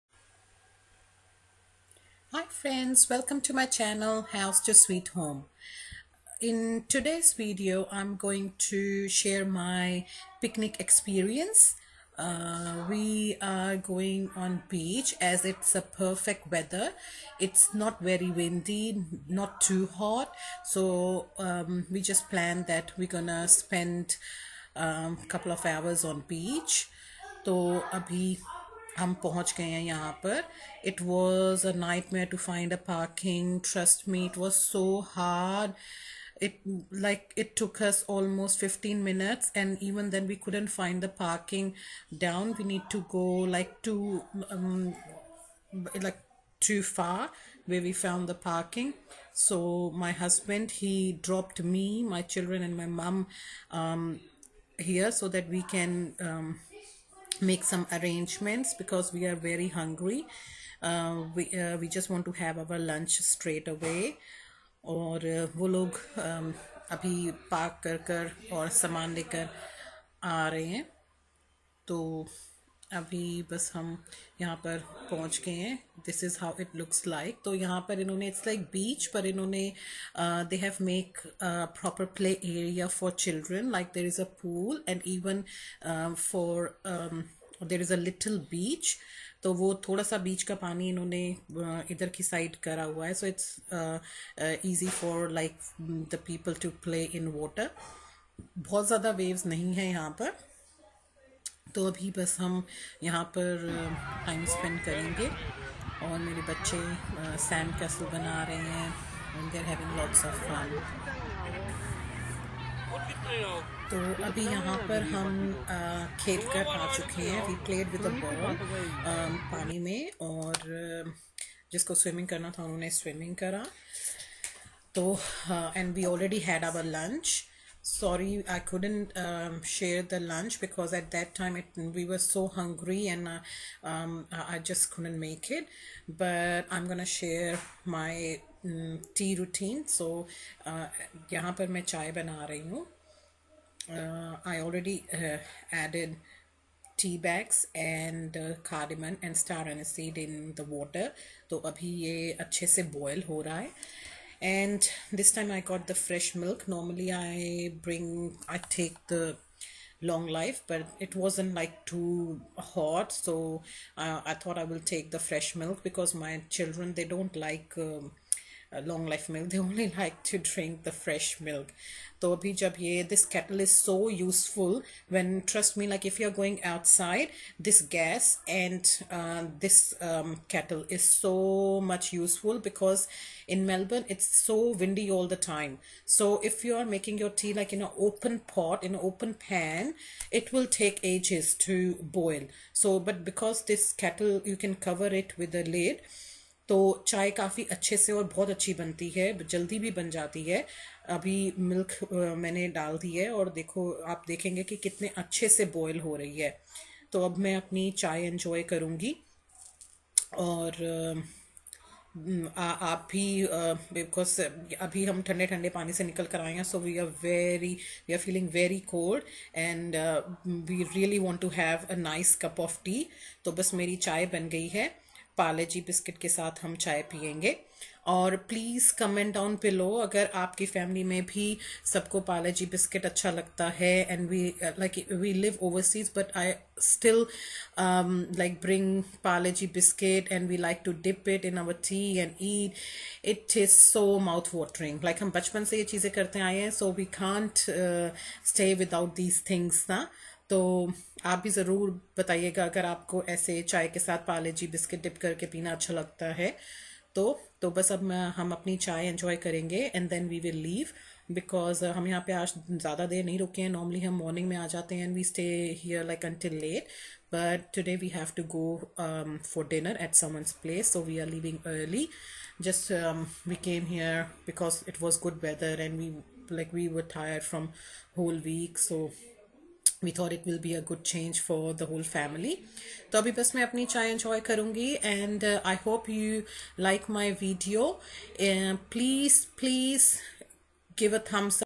Vlog: Indian NRI Mum, Having picnic on beach, Prepared fresh tea, Simple Living Wise Thinking
Hi everyone,
In this video i have shared my picnic day with you all. We ate our lunch there and after playing we prepared fresh masala tea.